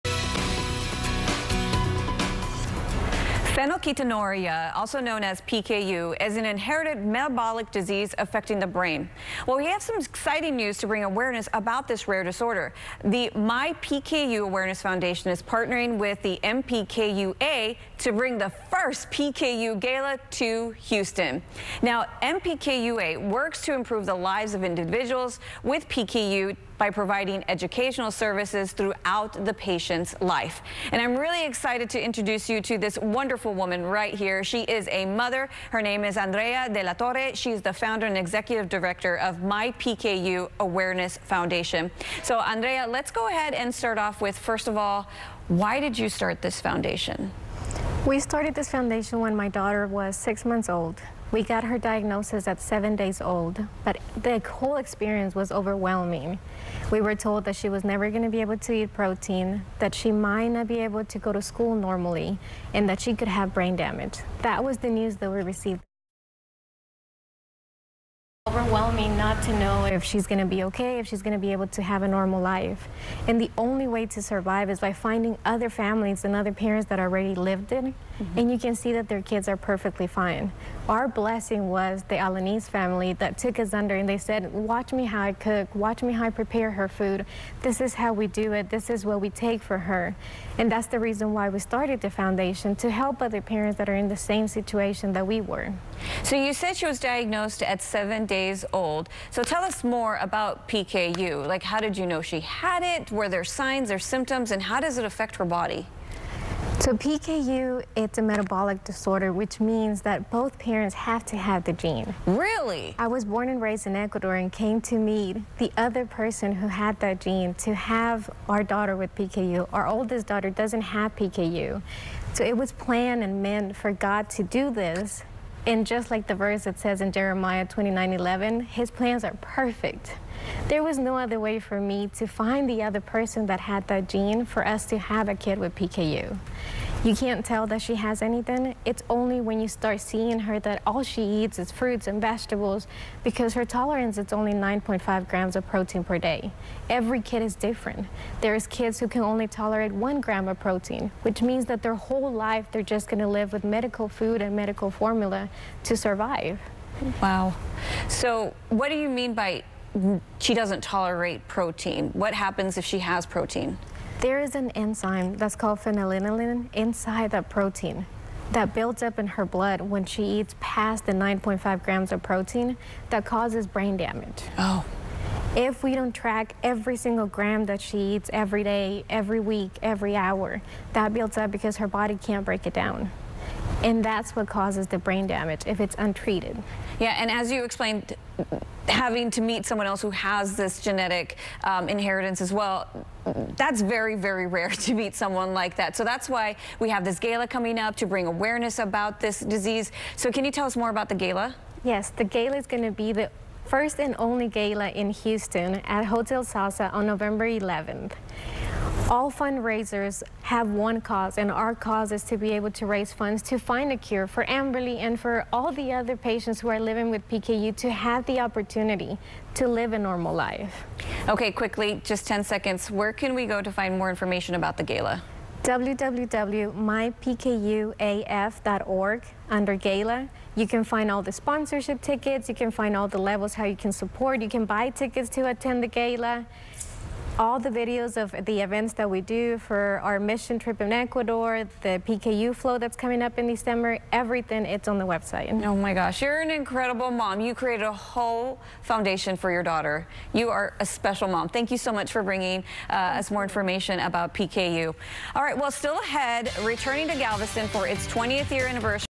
Rare disorder foundation to host first gala in Houston - Medical Minute, Idolina Peralez
Phenylketonuria, also known as PKU, is an inherited metabolic disease affecting the brain. The founder and executive director of the My PKU Awareness Foundation, Andrea De La Torre, stopped by the CW39 studios to discuss PKU and about the first PKU Gala in Houston.